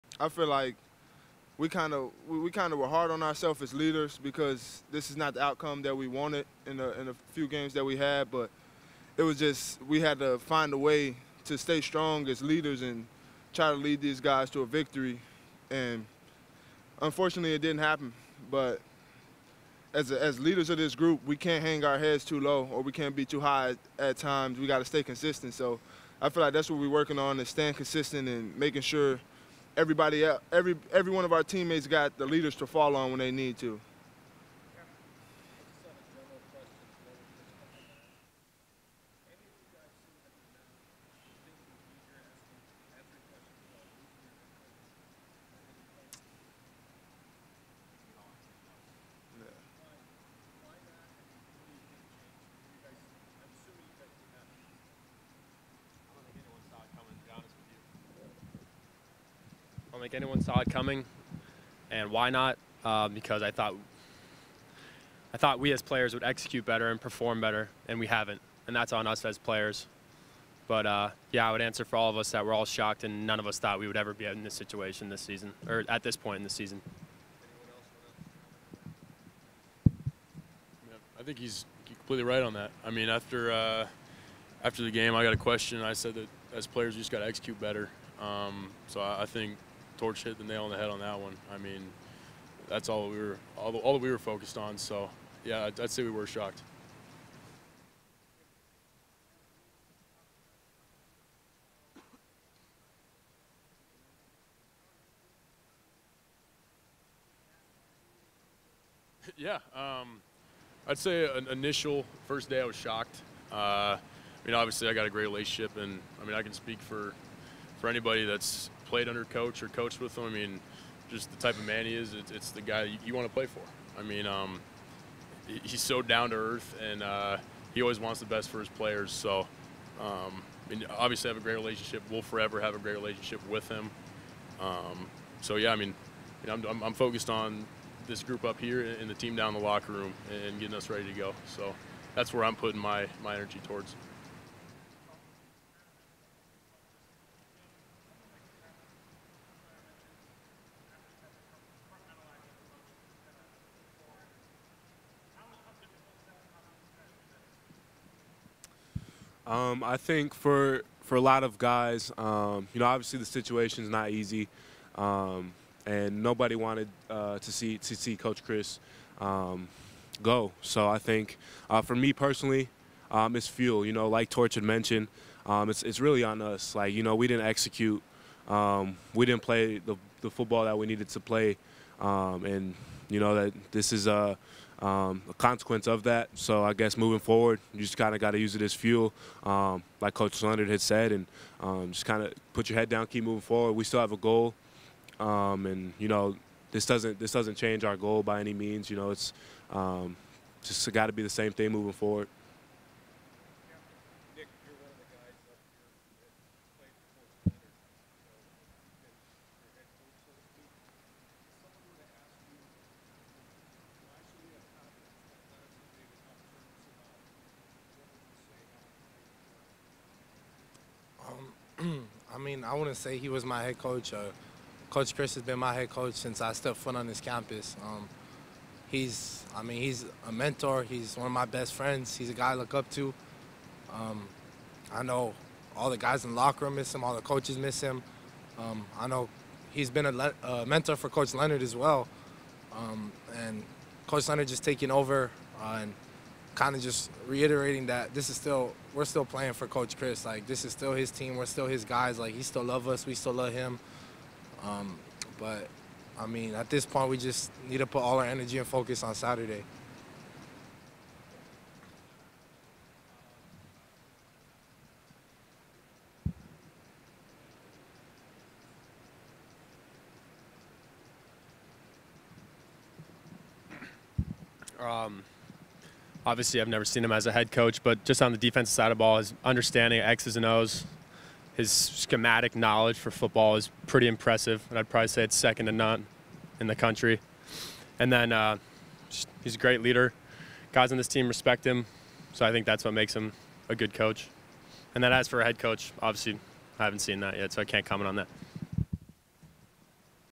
Badgers players respond to Paul Chryst firing - Part 1
Badgers players share their feelings after the sudden firing of head coach Paul Chryst after last weekend's loss to Illinois.